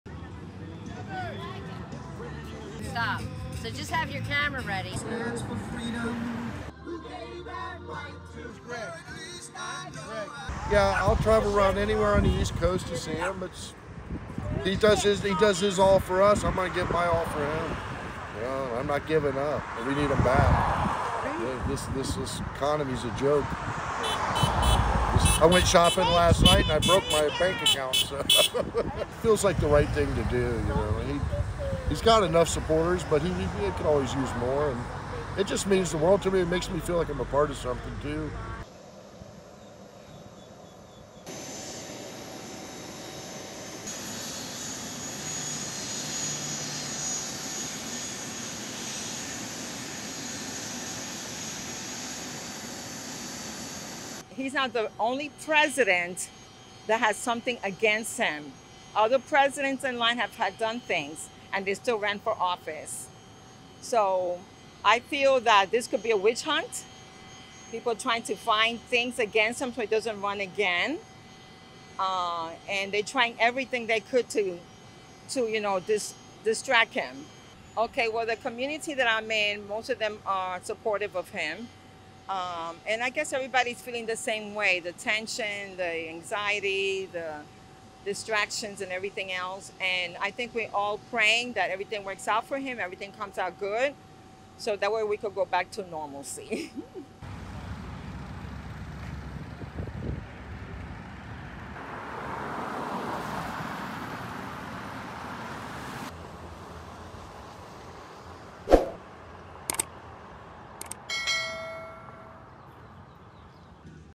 Trump supporters prepare for ex-President's departure from Florida residence.
Former U.S. President Donald Trump is set to make public remarks in Florida on Tuesday (April 4) after being arraigned in New York City on an indictment by a grand jury that heard evidence on hush money paid to a porn star before the 2016 election, while his lawyer said on Sunday he anticipates moving to dismiss the charges.

Trump, 76, is expected to be fingerprinted and photographed at a Manhattan courthouse as he becomes the first former U.S. president to face criminal charges.

On the third day after the indictment was announced, a handful of Trump supporters remained stationed on the Southern Blvd drawbridge across from Mar-a-Lago. The bridge has been a comradery point for several MAGA (Make America Great Again) supporters traveling from across the region.

Bringing you Truth, Inspiration, Hope.
Welcome to Vision Times News